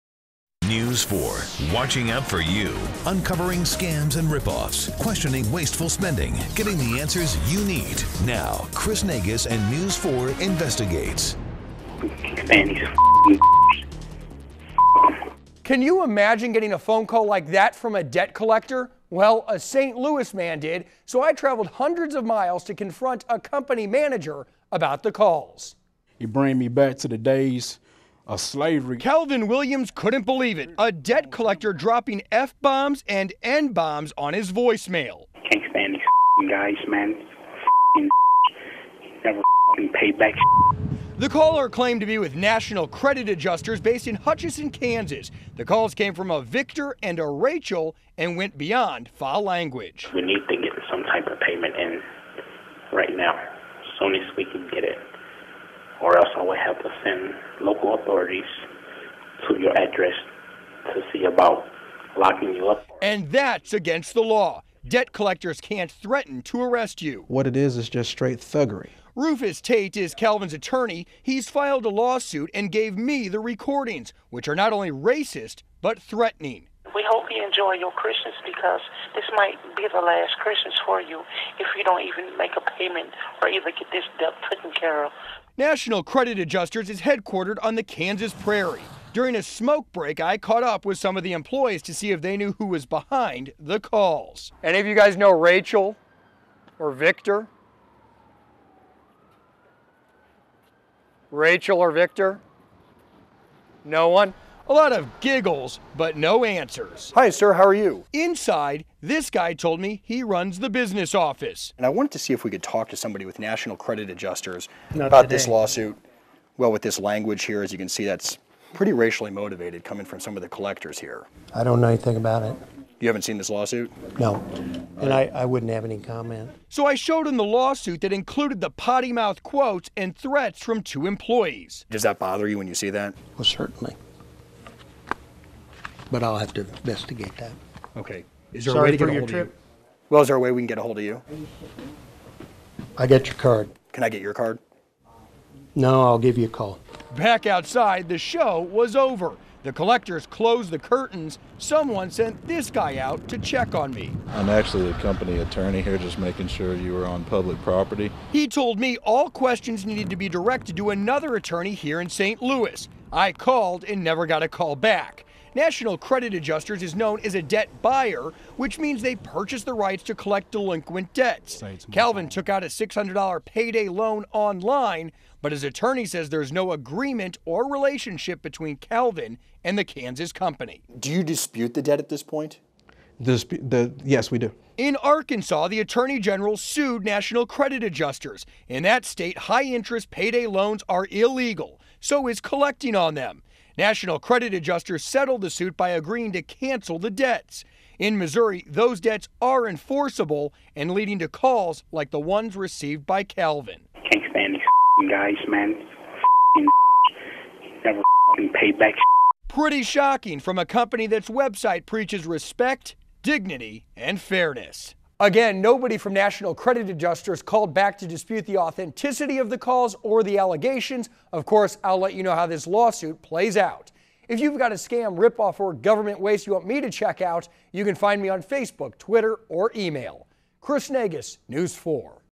National Credit Adjustors
A St. Louis man claims National Credit Adjustors made racially charged remarks during a debt collection call. News 4 traveled to Kansas to get answers from company management.